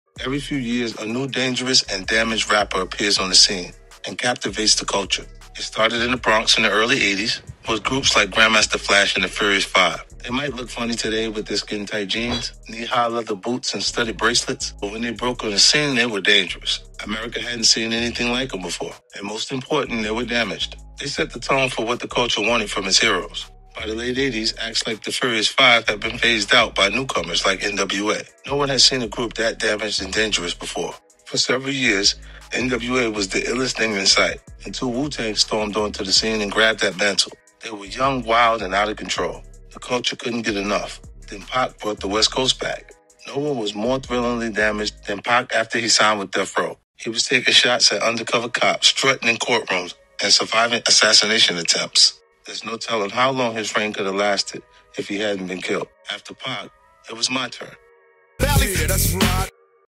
50 Cent, Wu-Tang, NWA and 2Pac: The Rise of Hip-Hop's Most Reckless Acts
In this DAILY SCOOP video, 50 Cent shares his thoughts on the evolution of dangerous and damaged rappers in hip-hop.

He begins by tracing the history of this phenomenon, starting with Grandmaster Flash & The Furious 5 in the early 80’s and moving through NWA, Wu-Tang, and Tupac.

Finally, he discusses how he took over as the reigning king of dangerous and damaged rap.

Throughout the video, he shares his insights on what makes these artists so captivating to the culture and what their impact has been on the genre as a whole.